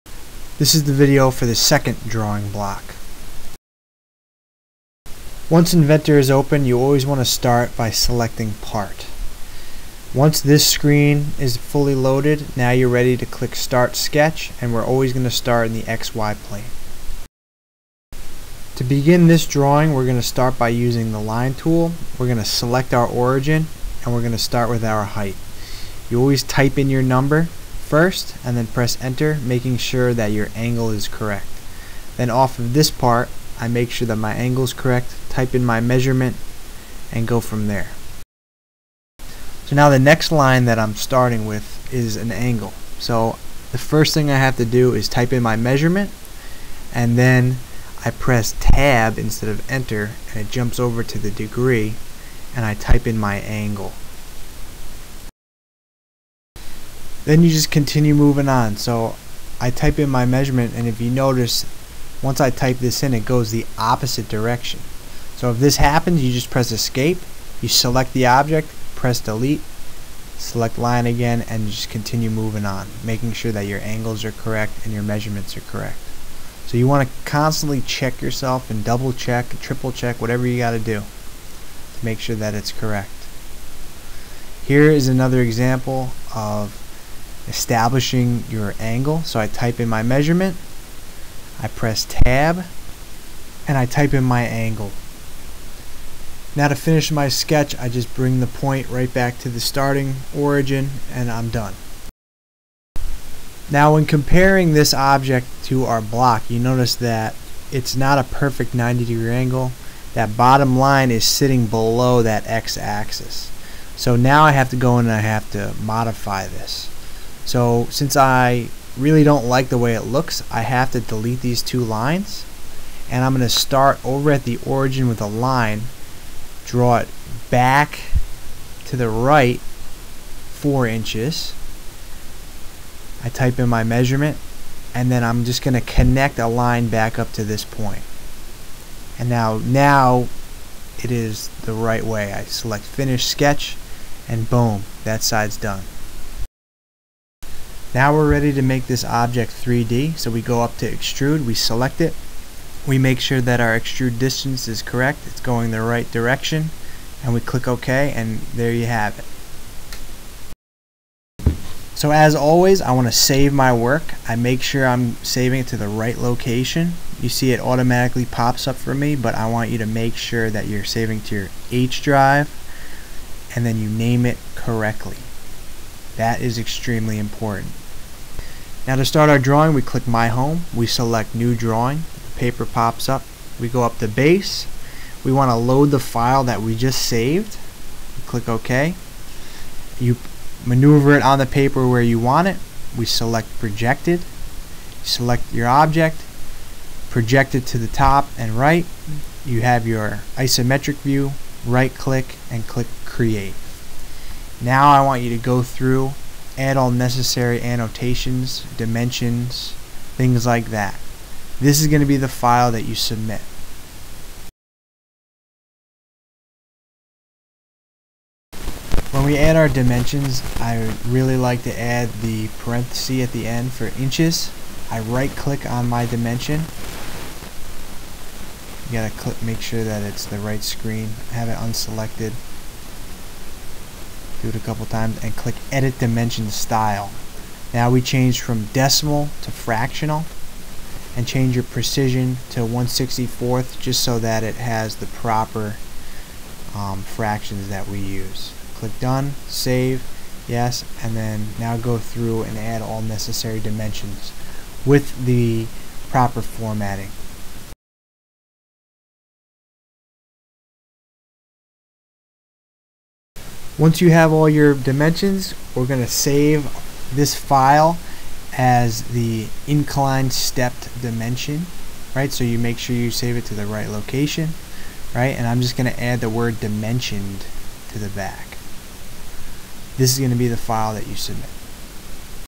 Inventor - Incline Stepped Block
This video goes over the basics tools to create a 3D shape with specific angles and reviews how to develop a multi-view drawing with annotations of a 3D shape